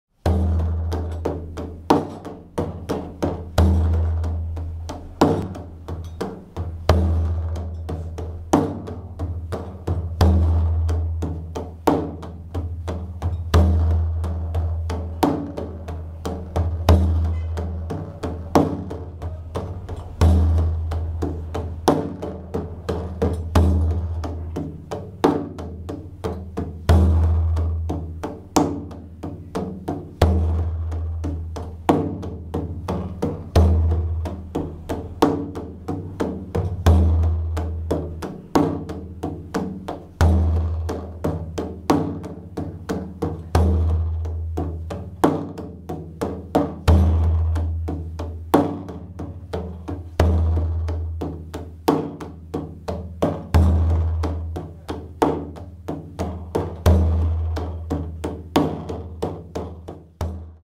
Jurjuna: D t k d m T t k d m
Variação que faz parte da sequência dos vídeos anteriores sobre a Jurjuna.
Consiste em tocar, alternando as mãos, o primeiro grupo de 5 começa com D na direita e o segundo com Tá na esquerda.

Para saber mais ou comprar um adufe em: adufes.com